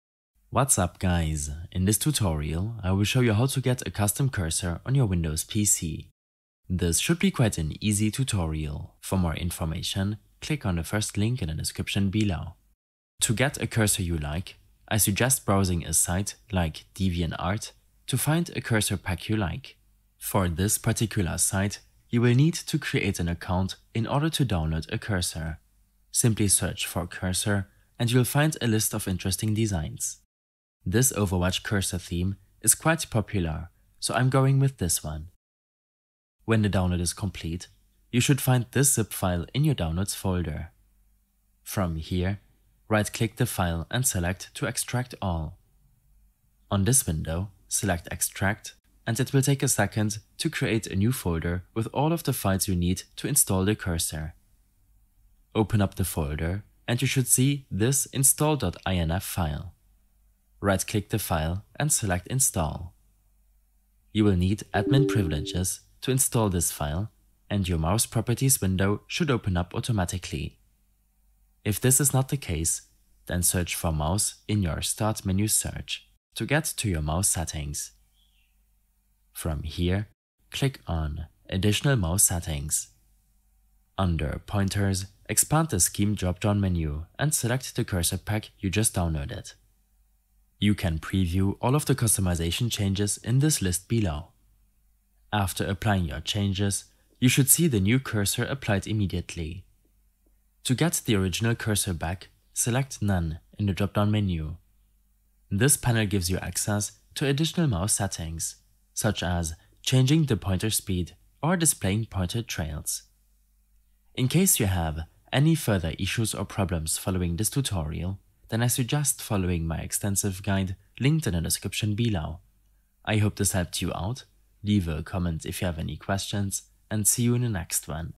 How To Get a Custom Windows Mouse Cursor
A short tutorial on how to get a custom cursor on your Windows PC.

Timestamps:
0:00 - Introduction
0:13 - Find Cursor
0:36 - Open ZIP File
0:55 - Install Cursor Pack
1:11 - Mouse Properties
1:23 - Choose New Cursor